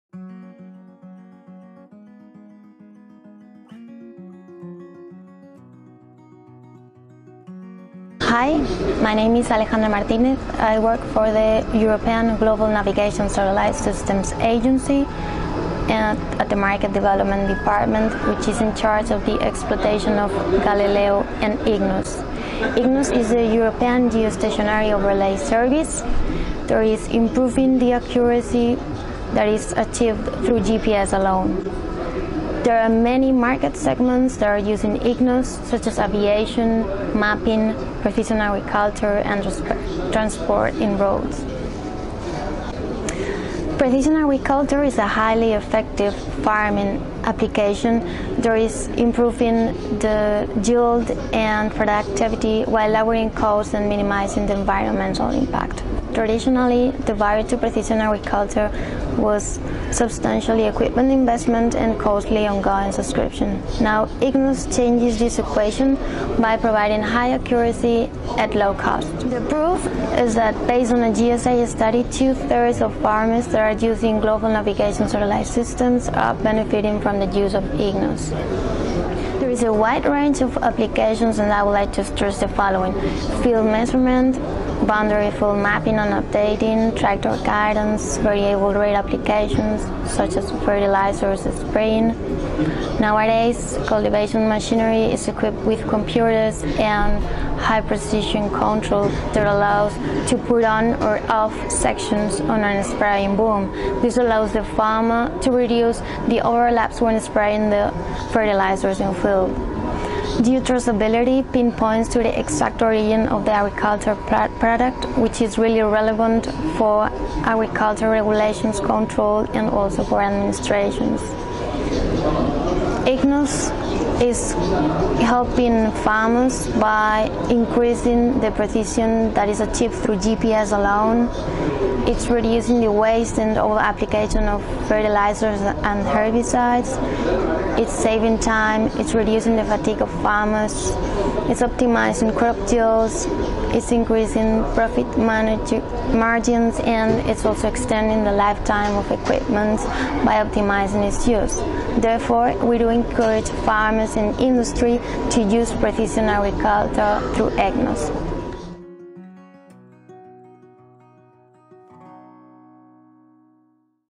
EGNOS and Precision Agriculture
How precision agriculture practice in Europe is benefiting from EGNOS. Watch this video taken at Geospatial World Forum 2014 in Geneva.